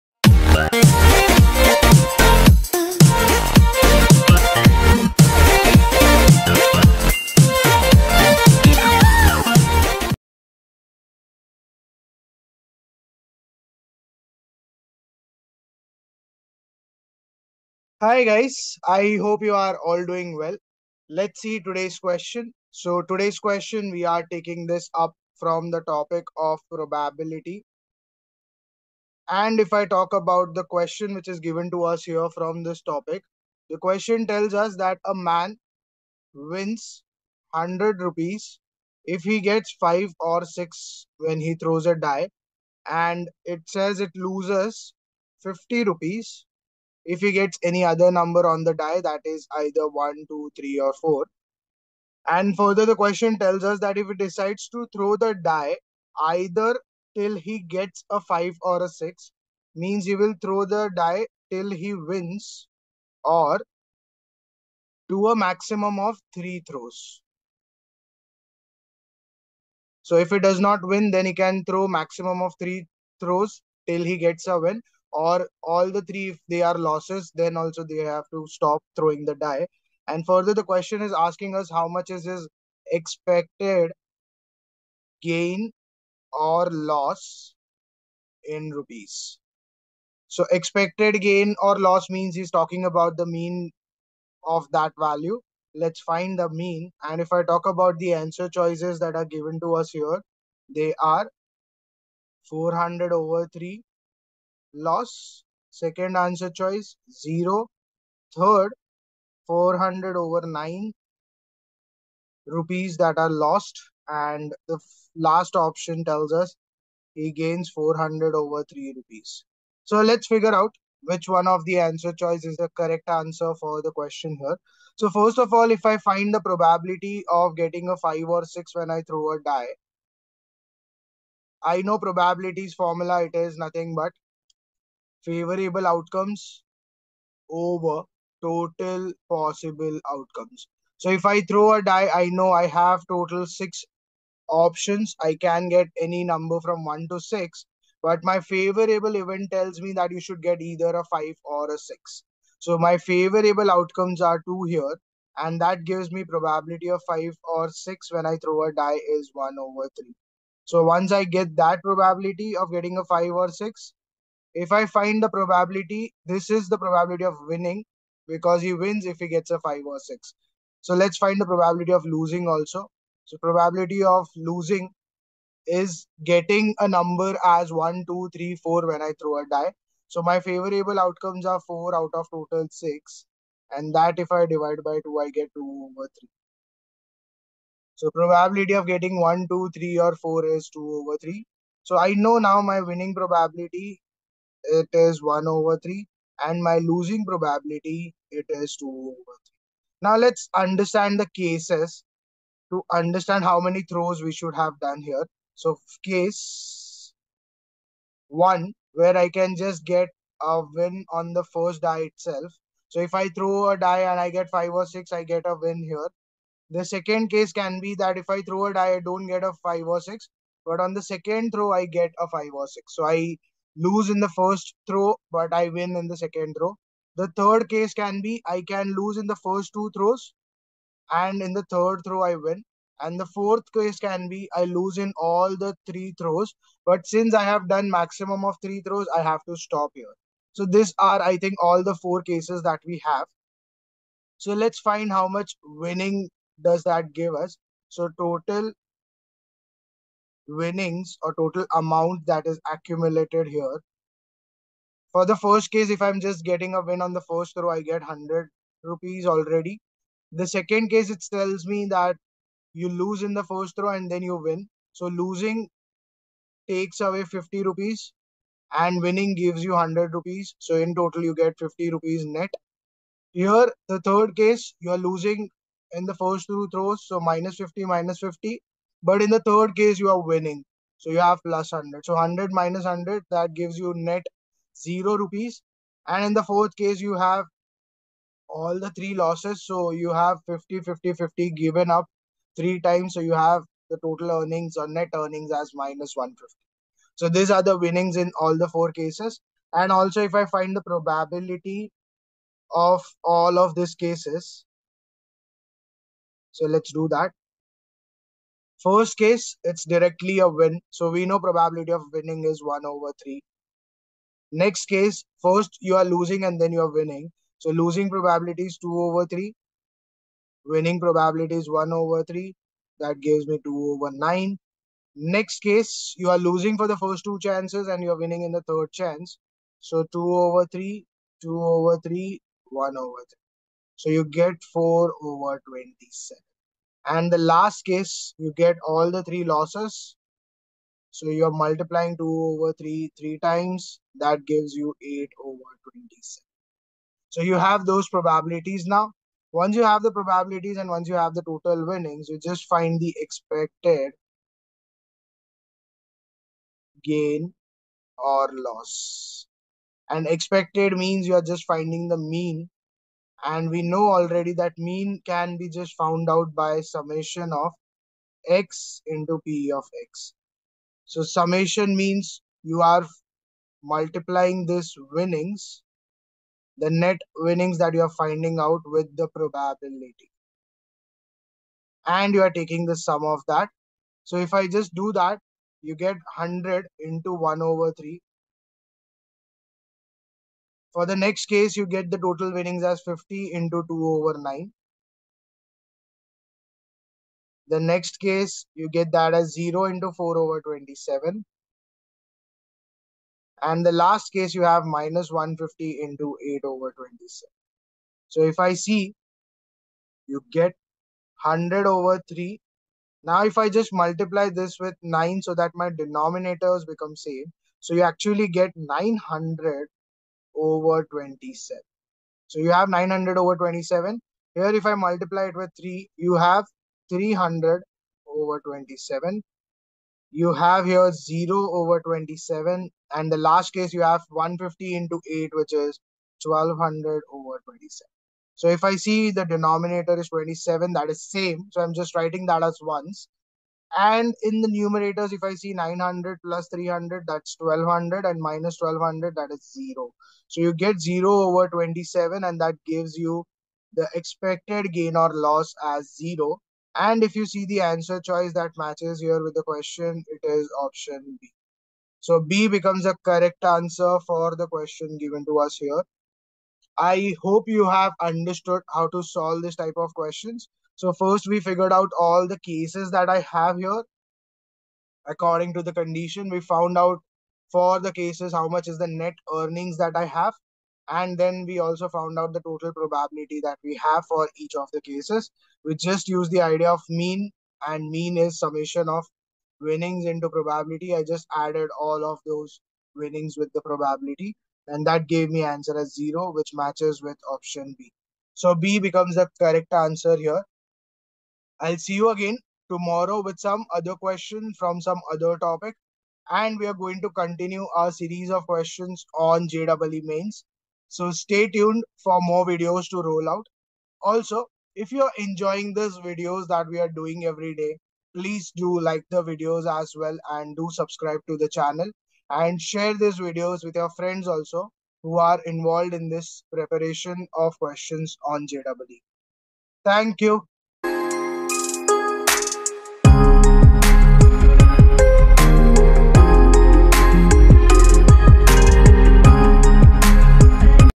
In a game, a man wins Rs.100 if he gets 5 or 6 on a throw of a fair die and loses Rs.50 for getting
In a game, a man wins Rs.100 if he gets 5 or 6 on a throw of a fair die and loses Rs.50 for getting any other number on the die. If he decides to throw the die either till he gets a five or a six or to a maximum of three throws, then his expected gain/loss(in rupees) is:
A. 400/3 Loss
B. 0
C. 400/9 loss
D. 400/3 gain